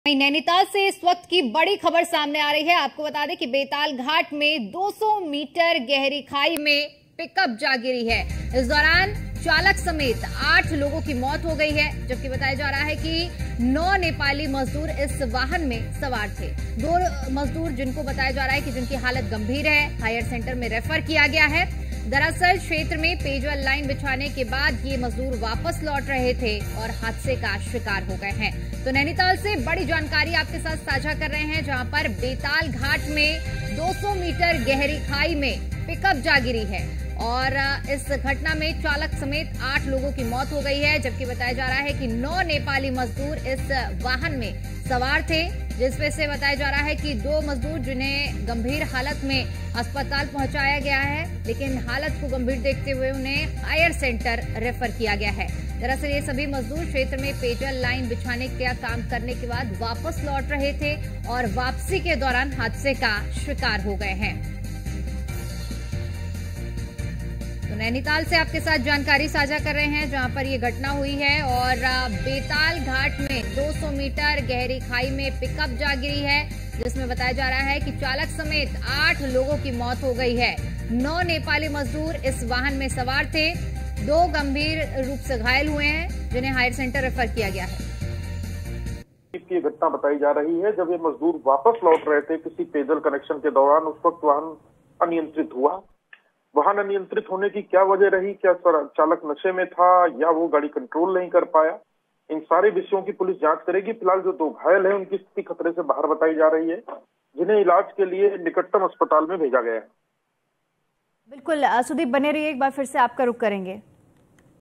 Nainital Accident News : नैनीताल में बड़ा हादसा, खाई में गिरा मैक्स वाहन, 8 लोगों की मौत | Uttraknad
Airtel - 350,
Netvision - 219,
RCN - 403,
UCN-223
Alisha Digital- 285
HDS- 127

We are also available on:
MX Player
JIO TV